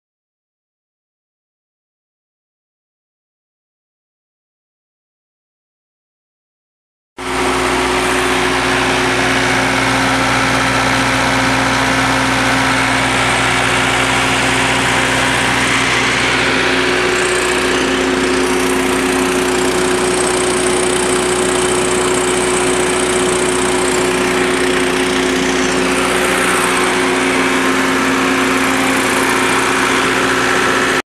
Miller portable welder/generator for sale | sold at auction December 31, 2014
Miller portable welder/generator
Four cylinder diesel engine
2" ball hitch
Manual jack
Safety chains
235/75R15 tires
No battery

It was located in Lexington, MO, and we sold it to the highest bidder for $1,210.